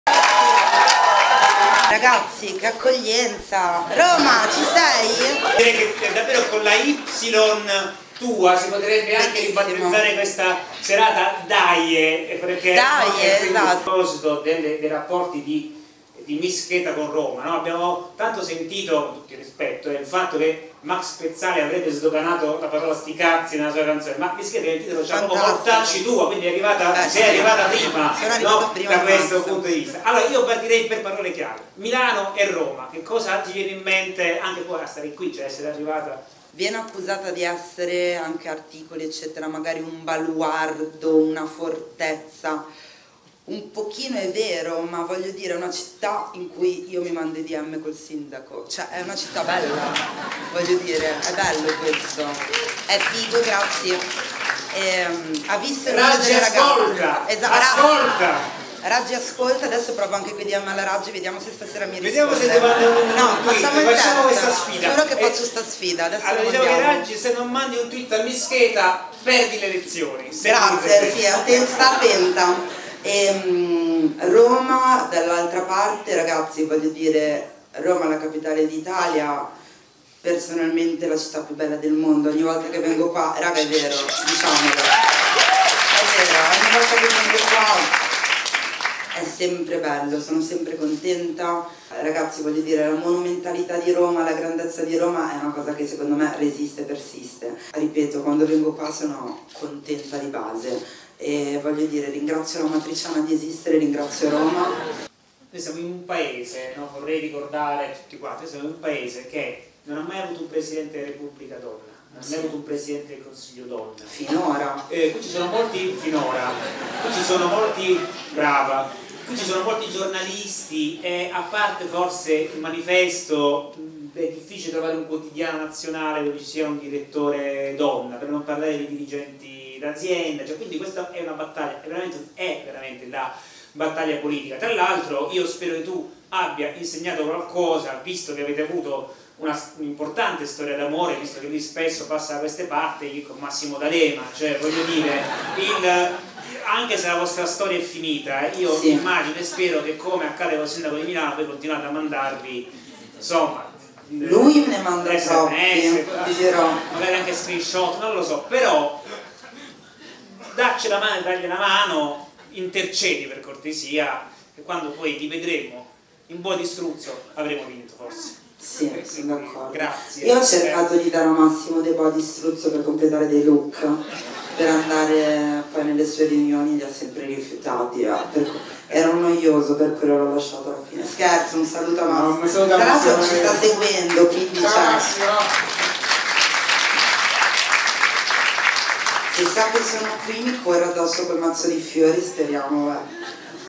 Myss Keta in trasferta a Roma: "Virginia, alle elezioni ti aiuto io"
"Tra Roma e Milano? Beh, con Beppe Sala ci scambiamo i 'dm' ('direct message', ndr) su Instagram, consiglio alla sindaca Raggi di scrivermi se vuole vincere le elezioni". Inizia così Myss Keta, prossima conduttrice de "L'altro Festival" sulla Rai, il parallelismo tra Roma e Milano incalzata dalle domande dello scrittore Nicola Lagioia al palazzo dell'Enciclopedia di Roma. E non può non parlare della sua 'love story' con Massimo D'Alema (con cui mesi fa si fece una foto proprio durante un suo concerto): "Se sa che sono qui mi corre dietro con un mazzo di fiori, lo conosco". .  .Video di Cristina Pantaleoni